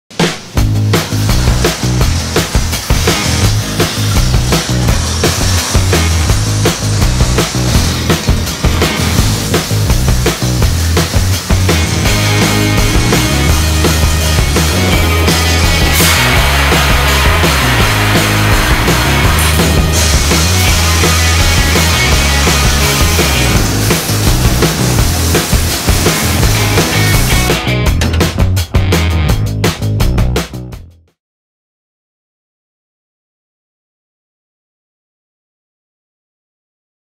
(ENG) FireWare Vesta in action
The FireWare Vesta is small and compact, but it generates a huge flame. The size of the flame is adjustable. The extinguisher trainer weighs 19 kilos and that makes it easy to carry. Ideal when you need to set up a training by yourself!